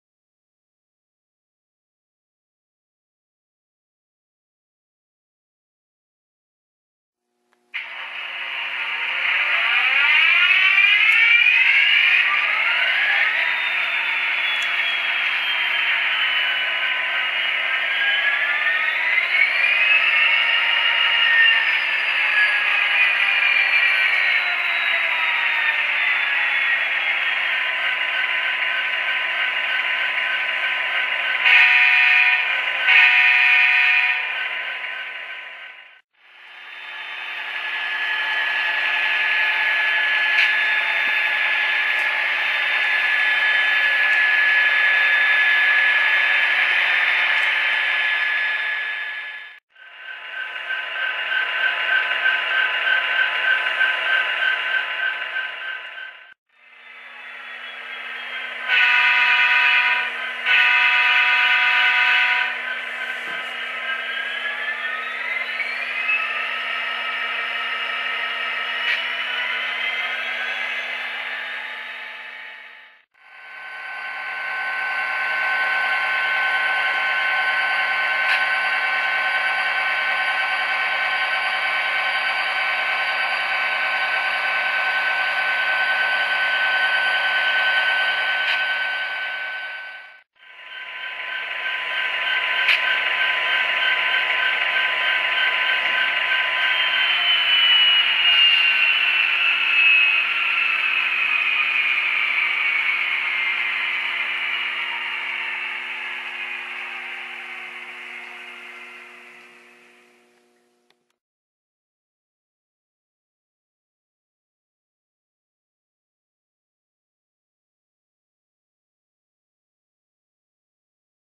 Custom Detailed Proto 2000 DRGW GP60 #3156
Custom detailed Walthers Proto 2000 (new run) Rio Grande GP60 #3156 with factory Tsunami sound. I added prototype correct details, upgraded the speaker and installed LED ditch lights.

eBay store